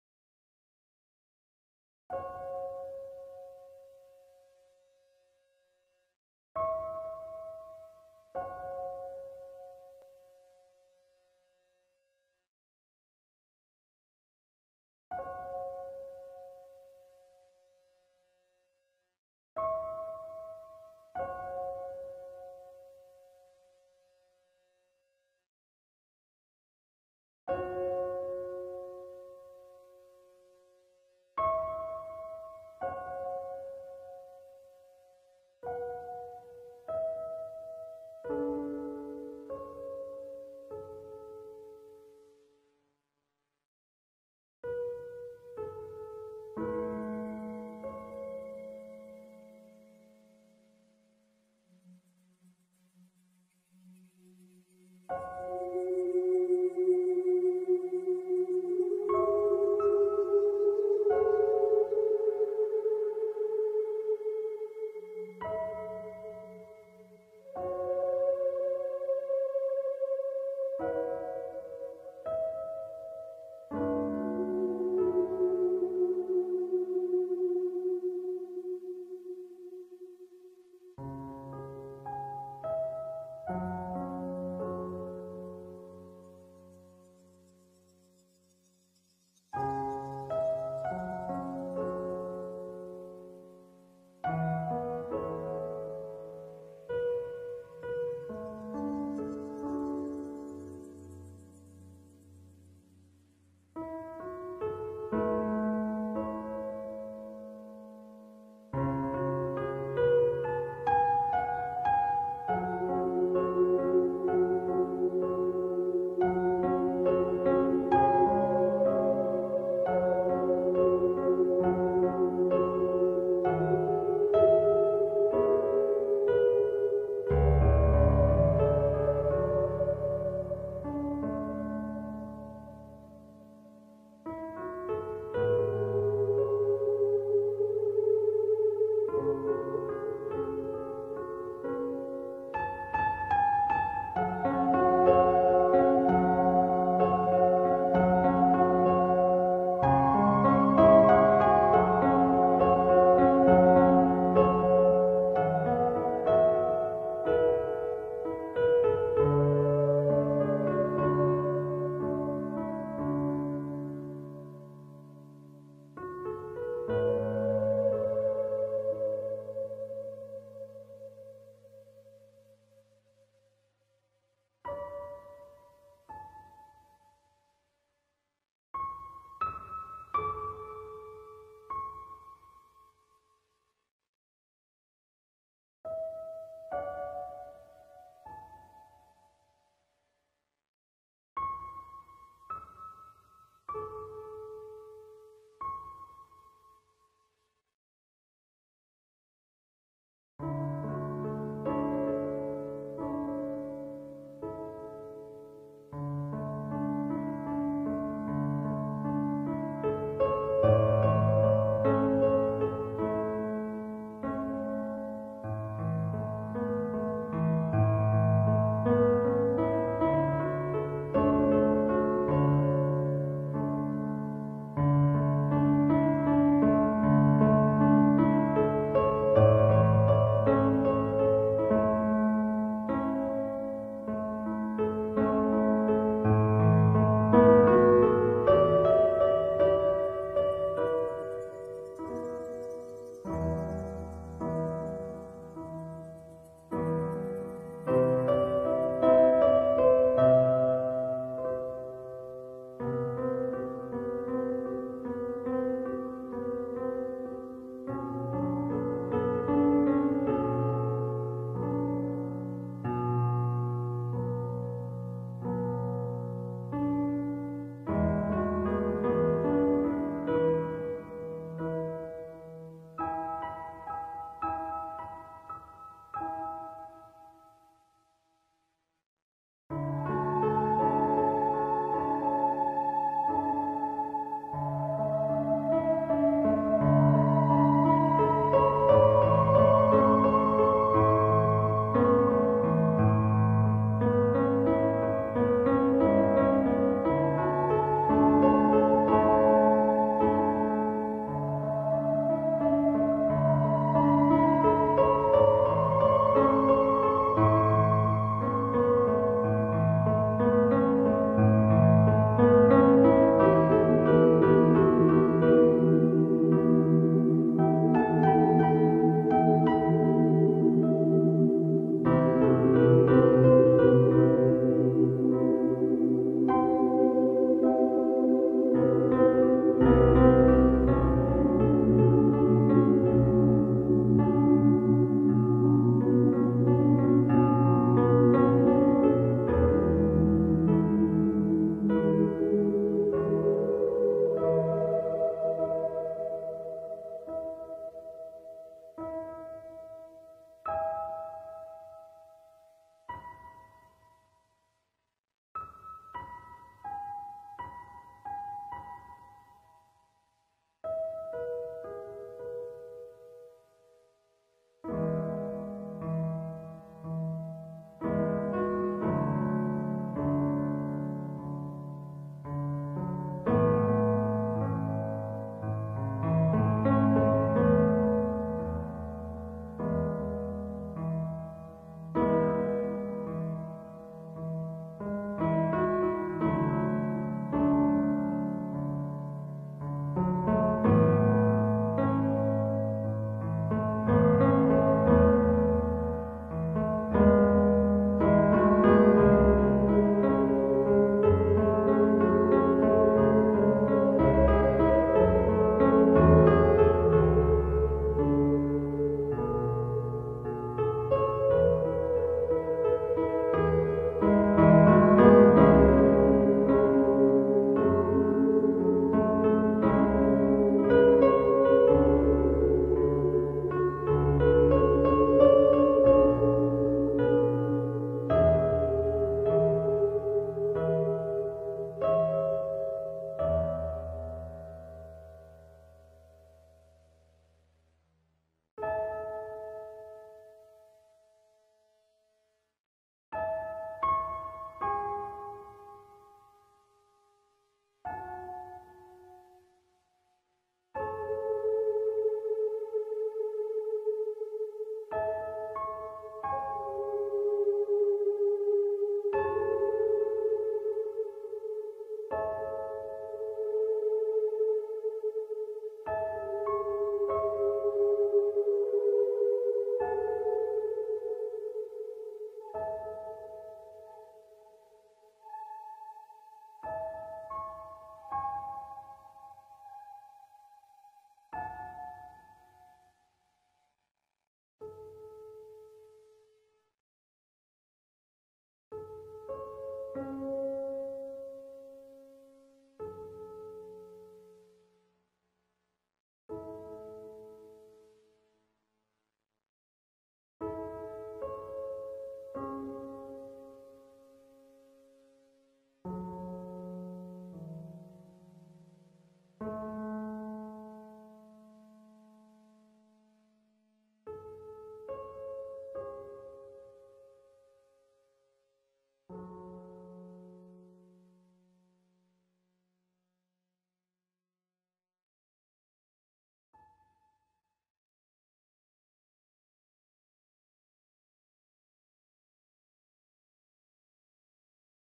Easy sunset speed painting with watercolors for beginners
Hi everyone) This is my easy sunset painting with watercolor for beginners) I hope it will be useful for you) Enjoy!)